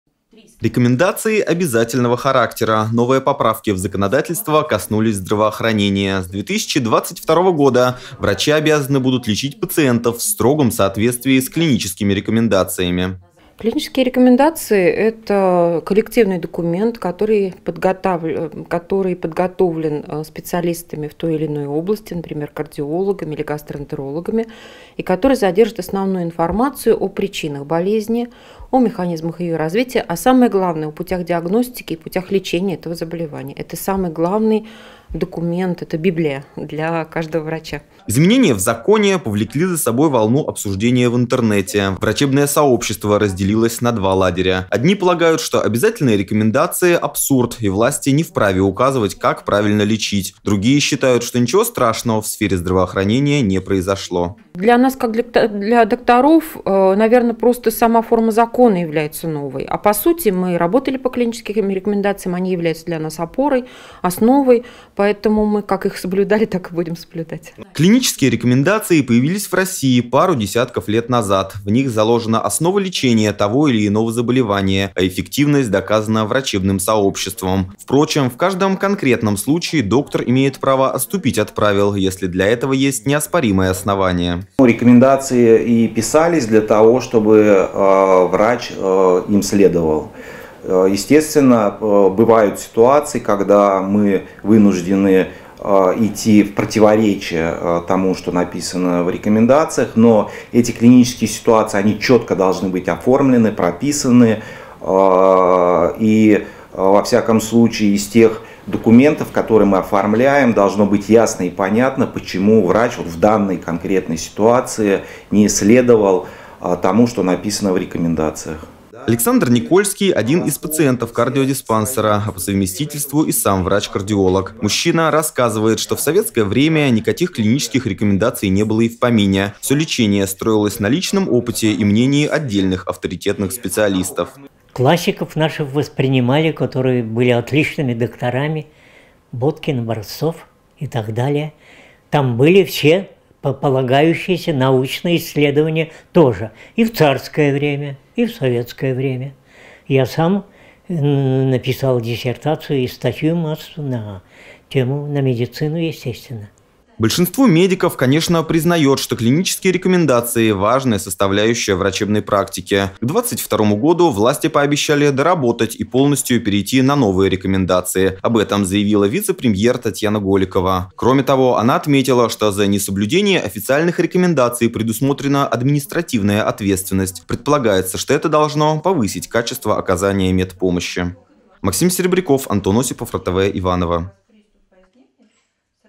Лечить строго по плану!
Рекомендации обязательного характера. Новые поправки в законодательство коснулись здравоохранения. С 2022 года врачи обязаны будут лечить пациентов в строгом соответствии с клиническими рекомендациями. Как это отразится на отечественной медицине, рассказали в ивановском кардиологическом диспансере.